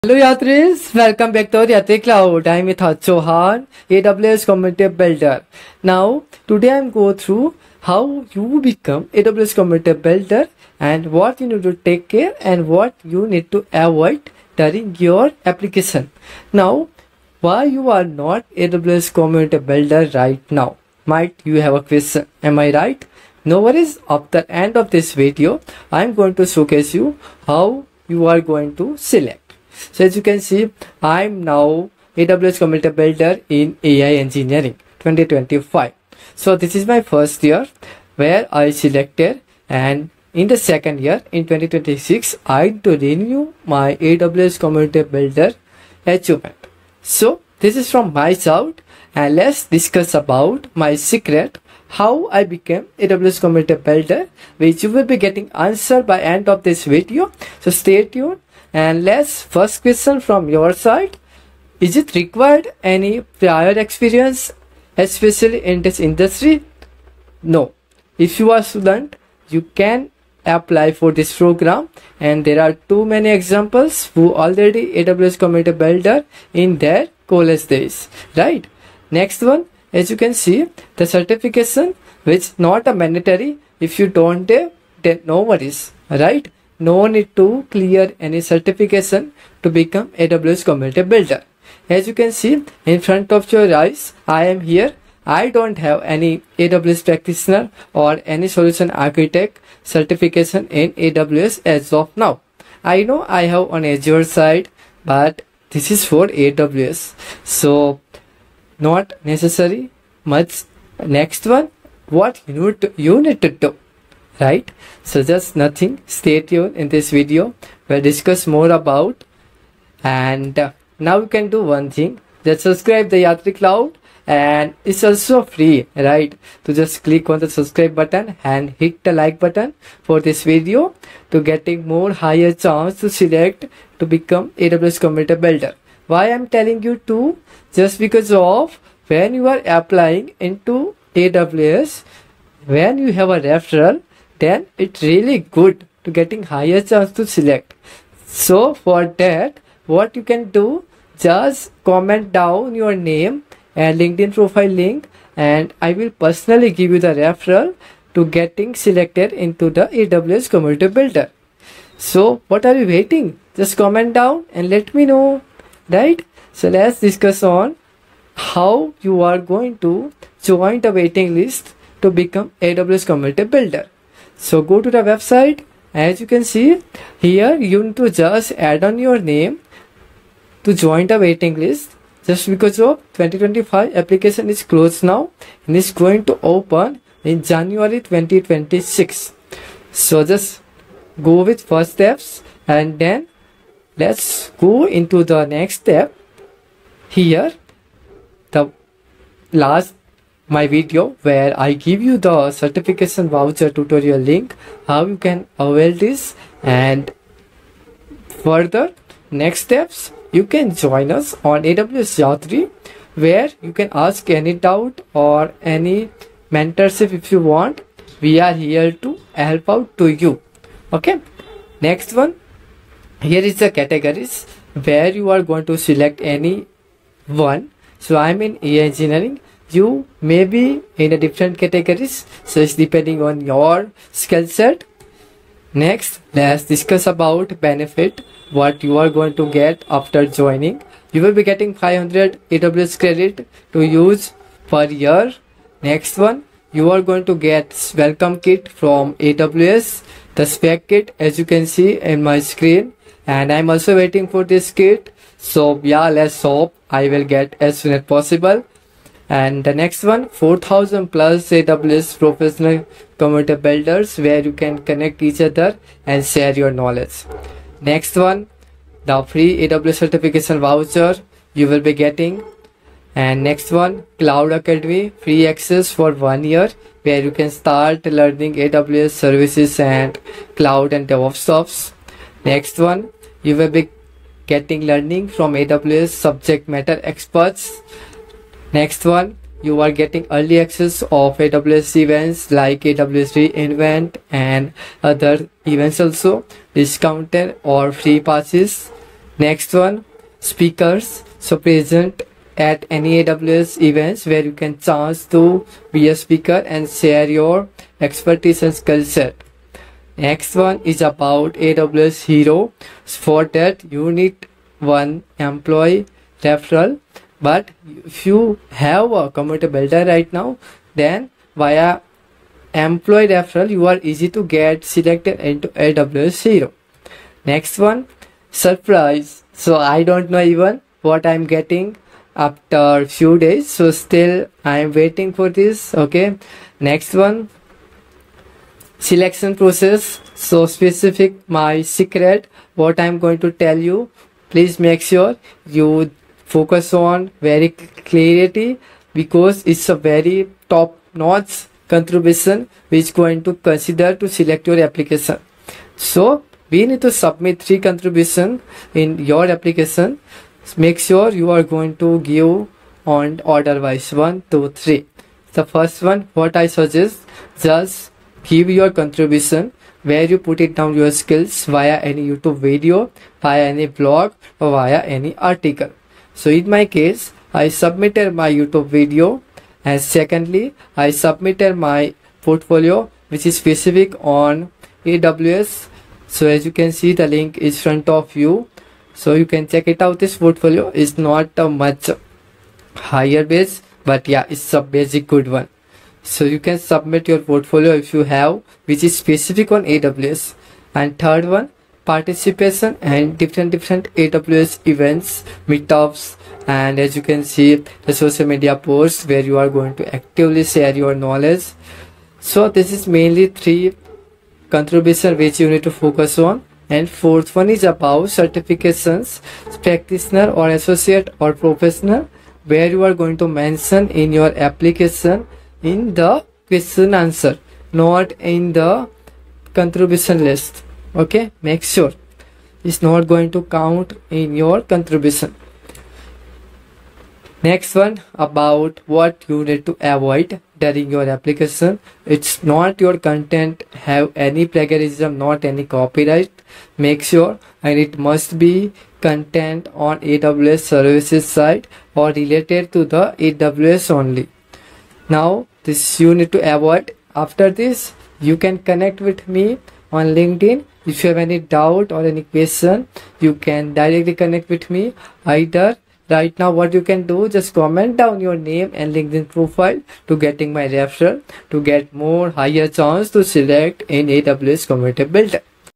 My SECRET: How to Become an AWS Community Builder 2025 | Benefits and How to Apply | Complete Guide
Want to join the AWS Community Builder Program and grow your career in cloud?

In this video, I’ll share my secret tips on how to apply, the benefits, and what you need to increase your chances of getting selected!

💬 Drop your Name + LinkedIn profile to get my Referral in the comments!

✅ What You’ll Learn:
✔️ Step-by-step application process
✔️ Tips to stand out
✔️ AWS Community Builder benefits